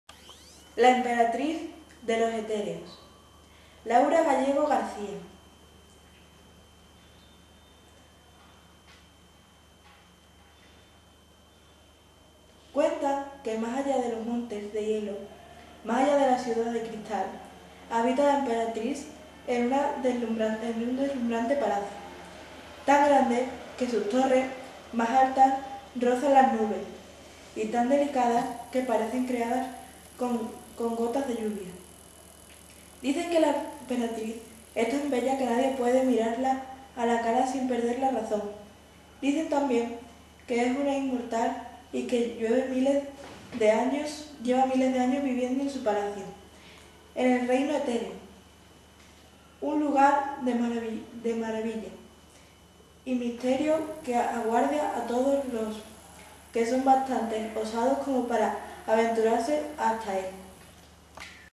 jose andres - la emperatriz de los etereos
Videomaratón de lectura de la provincia de Sevilla - El Placer de Leer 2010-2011.
José Andrés, desde el "Club de Lectura Juvenil El Pedroso" de El Pedroso nos lee un fragmento de "La emperatriz de los etereos".